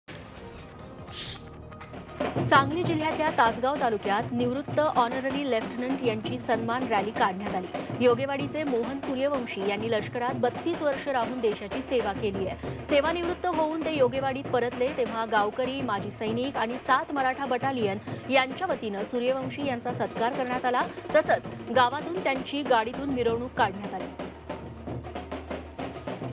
Sangali । सांगली जिल्ह्यातल्या तासगाव तालुक्यात निवृत्त ऑनररी लेफ्टनंट यांची सन्मान रॅली
सांगली जिल्ह्यातल्या तासगाव तालुक्यात निवृत्त ऑनररी लेफ्टनंट यांची सन्मान रॅली काढण्यात आली. योगेवाडीचे मोहन सूर्यवंशी यांनी लष्करात 32 वर्ष राहून देशाची सेवा केलीय. सेवानिवृत्त होऊन ते योगेवाडीत परतले. तेव्हा गावकरी, माजी सैनिक आणि सात मराठा बटालियन यांच्या वतीनं सूर्यवंशी यांचा सत्कार करण्यात आला. तसंच त्यांची गाडीतून मिरवणूक काढण्यात आली.

5 Minitat 25 Batmya । 5 मिनिटांत 25 बातम्या